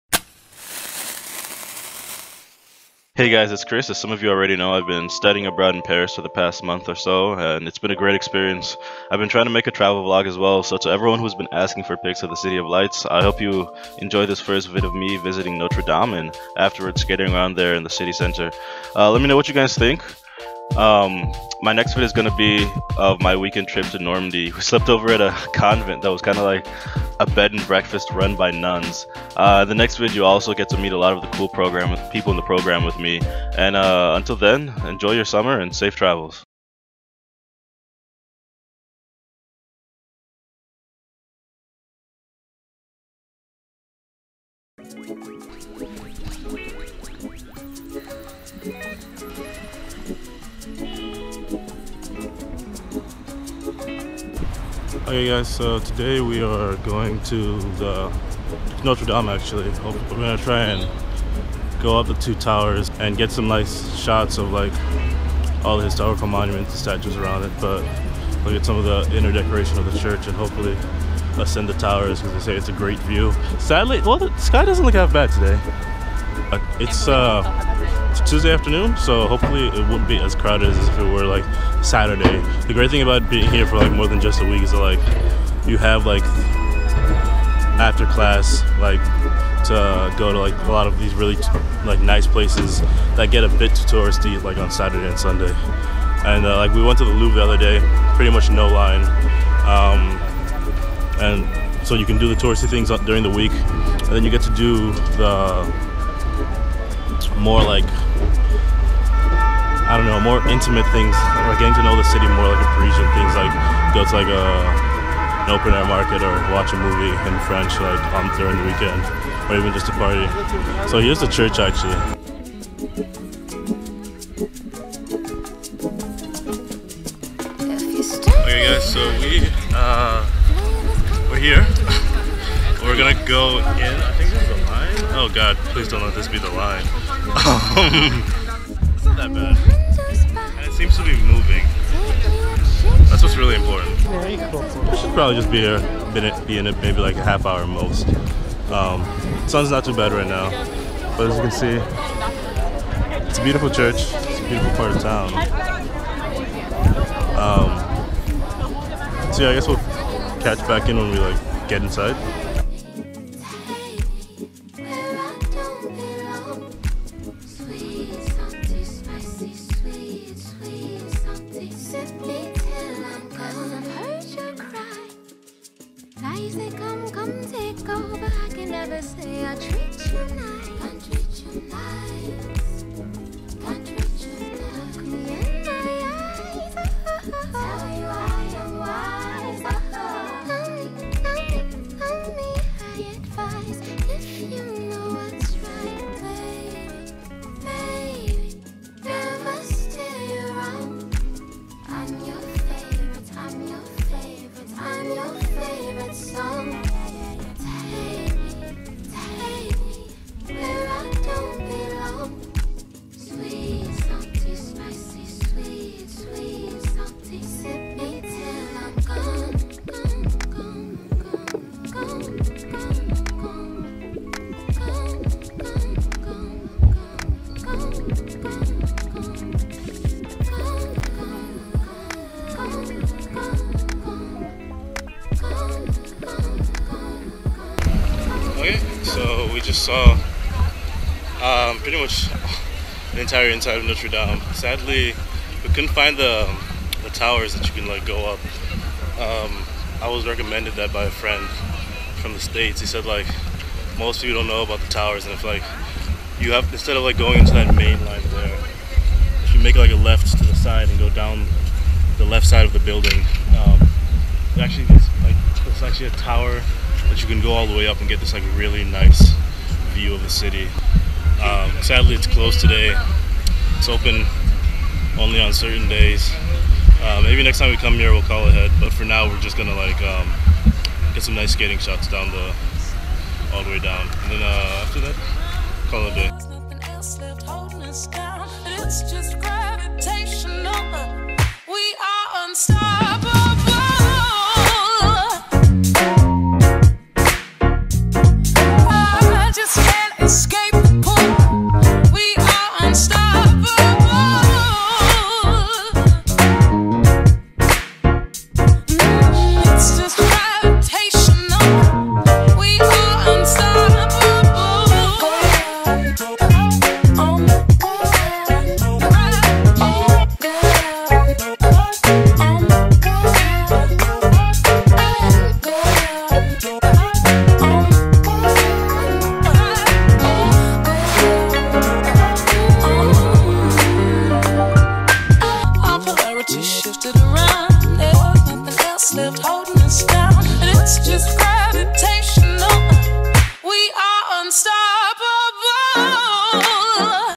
Paris Vlog: Visiting Notre Dame and Skateboarding Around Central Paris - #1
My trip to Paris has been great and I've got so many experiences to bring back. Check out week one my early visits to Notre Dame and with a skate sesh around central Paris. Keep an eye out for more vids from this series.